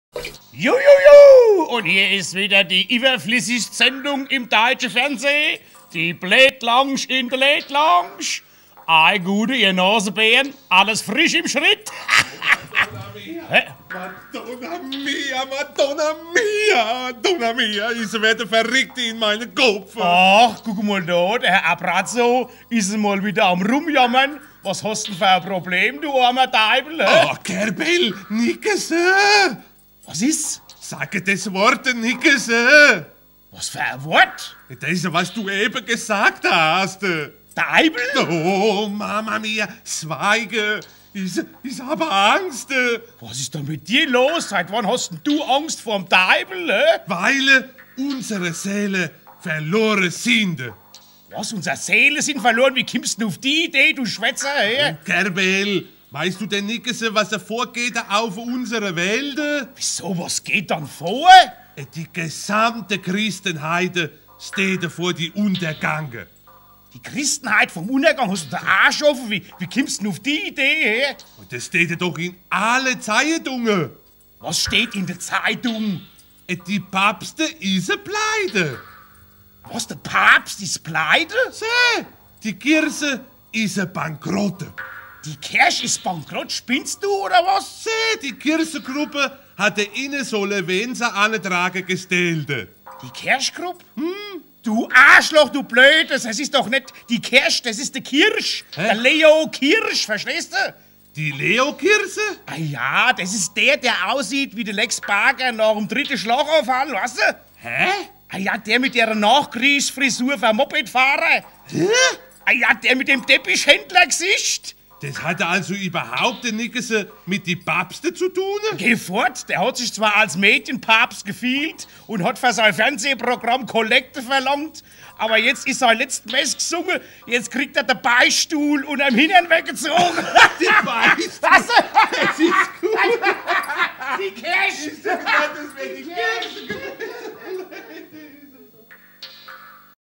Leo Kirch
Die Ratten Körbel und Abrazzo aus dem Kikeriki Theater in ihrer ersten eigenen Late Night Show.